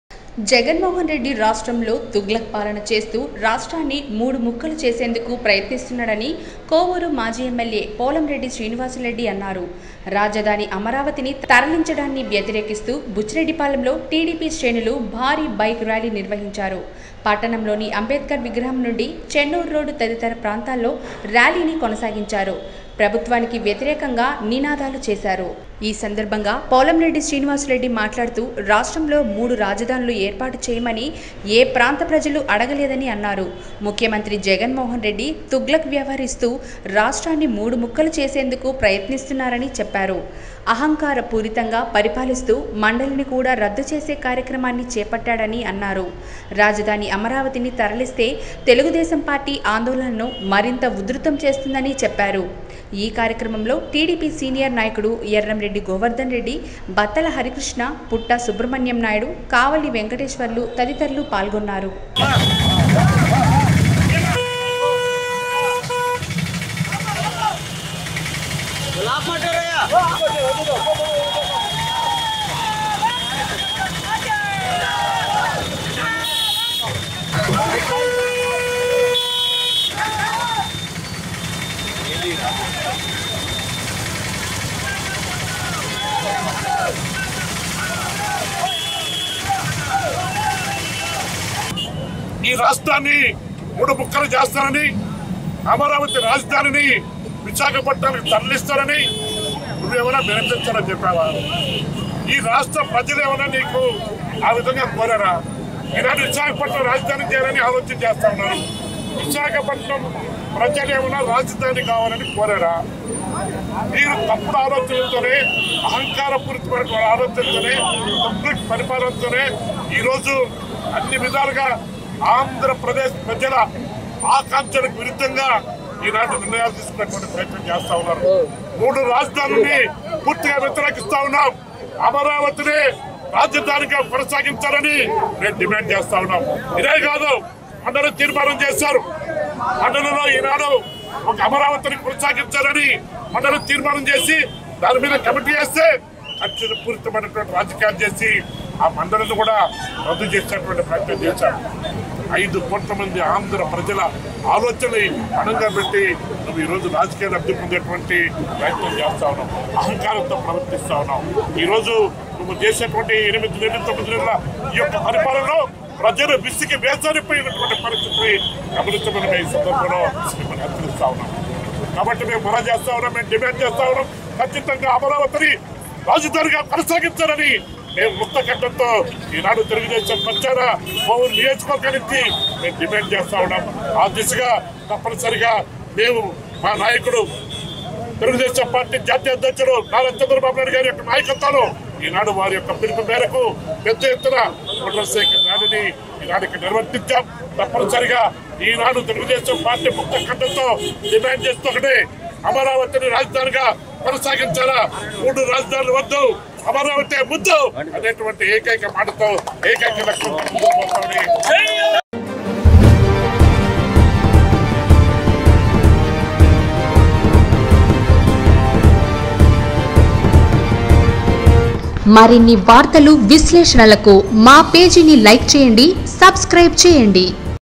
మూడు రాజధానులు వద్దు-- అమరావతి ముద్దు ---పోలంరెడ్డి శ్రీనివాసులురెడ్డి - Simhapuri News
మూడు రాజధానులు వద్దు-- అమరావతి ముద్దు ---పోలంరెడ్డి శ్రీనివాసులురెడ్డి
జగన్మోహన్ రెడ్డి రాష్ట్రంలో తుగ్లక్ పాలన చేస్తూ రాష్ట్రాన్ని మూడు ముక్కలు చేసేందుకు ప్రయత్నిస్తున్నాడని కోవూరు మాజీ ఎమ్మెల్యే పోలంరెడ్డి శ్రీనివాసులురెడ్డి అన్నారు. రాజధాని అమరావతినీ తరలించడాన్ని వ్యతిరేకిస్తూ బుచ్చిరెడ్డిపాలెం లో టిడిపి శ్రేణులు భారీ బైక్ ర్యాలీ నిర్వహించారు. పట్టణంలోని అంబేద్కర్ విగ్రహం నుండి చెన్నూర్ రోడ్డు తదితర ప్రాంతాల్లో ర్యాలీని కొనసాగించారు. ప్రభుత్వానికి వ్యతిరేకంగా నినాదాలు చేశారు
.ఈ సందర్భంగా పోలంరెడ్డి శ్రీనివాసులురెడ్డి మాట్లాడుతూ రాష్ట్రంలో మూడు రాజధానులు ఏర్పాటు చేయమని ఏ ప్రాంత ప్రజలు అడగలేదని అన్నారు. ముఖ్యమంత్రి జగన్మోహన్రెడ్డి తుగ్లక్ వ్యవహరిస్తూ రాష్ట్రాన్ని మూడు ముక్కలు చేసేందుకు ప్రయత్నిస్తున్నారని చెప్పారు. అహంకార పూరితంగా పరిపాలిస్తూ  మండలిని కూడా రద్దు చేసే కార్యక్రమాన్ని చేపట్టాడని అన్నారు. రాజధాని అమరావతినీ తరలిస్తే తెలుగుదేశం పార్టీ ఆందోళనను మరింత ఉధృతం  చేస్తుందని చెప్పారు. ఈ కార్యక్రమంలో టిడిపి సీనియర్ నాయకుడు ఎర్రంరెడ్డి గోవర్ధన్ రెడ్డి, బత్తల హరికృష్ణ, పుట్టా సుబ్రహ్మణ్యం నాయుడు, కావలి వెంకటేశ్వర్లు తదితరులు పాల్గొన్నారు